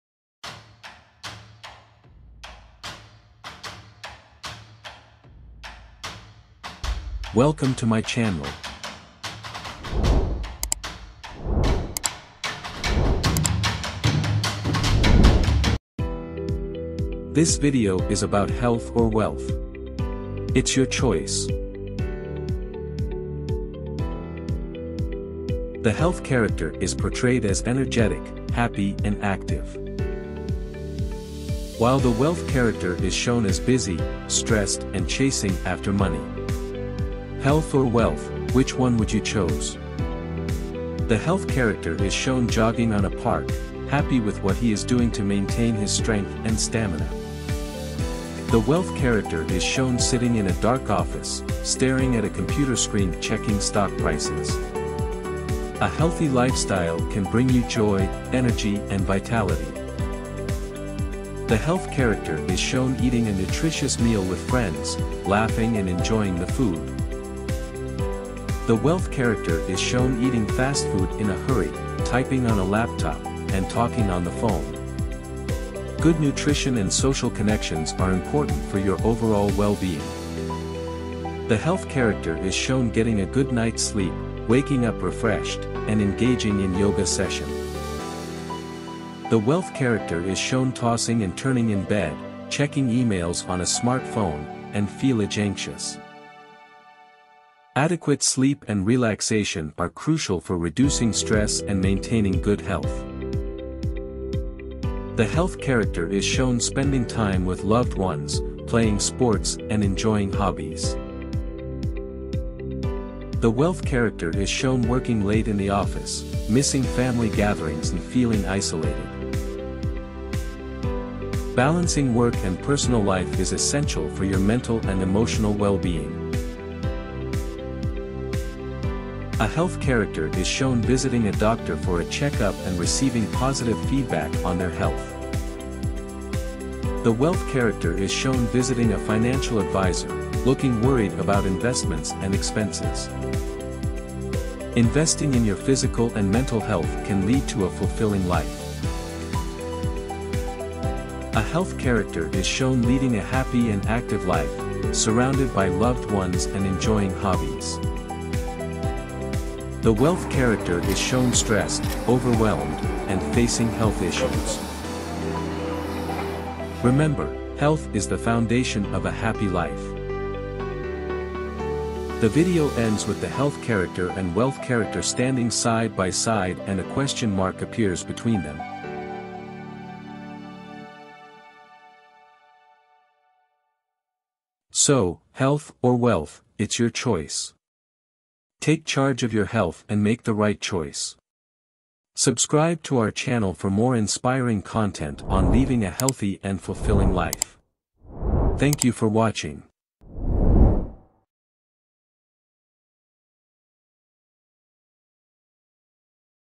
Health or Wealth, It's Your Choice
This video was made with Clipchamp. This video is a captivating and thought provoking video that explores the age-old question of what matters most in life- health or wealth.
Through contrasting scenes, the video showcases how a healthy lifestyle can lead to a fulfilling life, while neglecting health in pursuit of wealth may result in adverse consequences. The characters' stories culminate in a striking reminder that health is the foundation of a happy life, leaving viewers with a question mark, prompting them to reflect on their own choices.